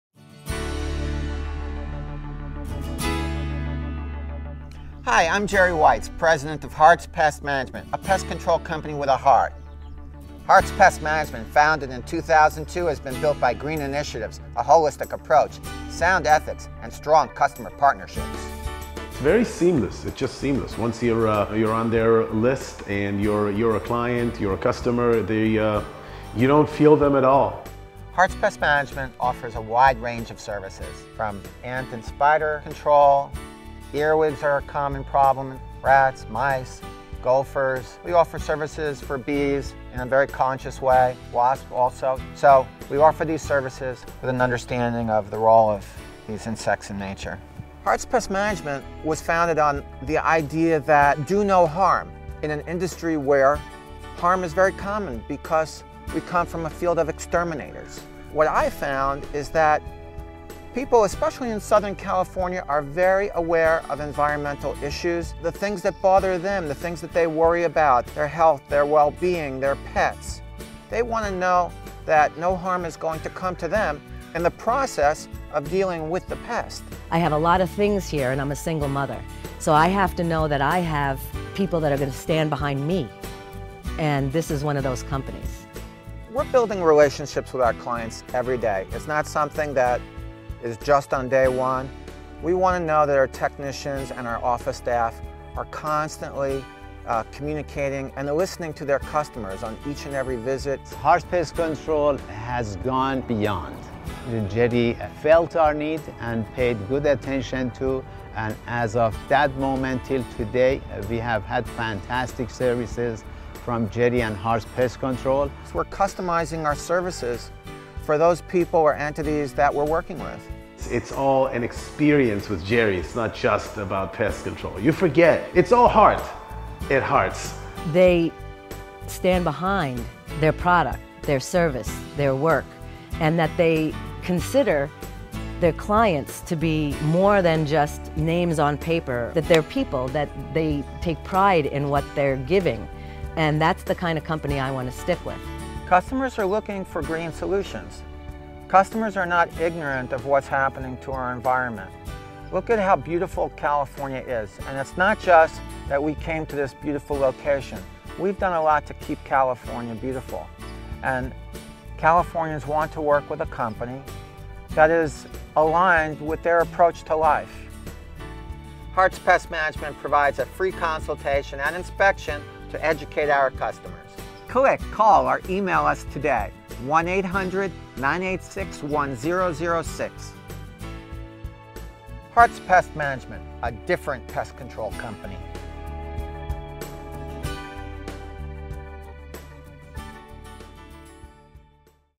Hearts Pest Management Profile Video
Hearts Pest Management is presented in eyes of owner, Gerry Weitz, as well as three diverse customers for residential and commercial services.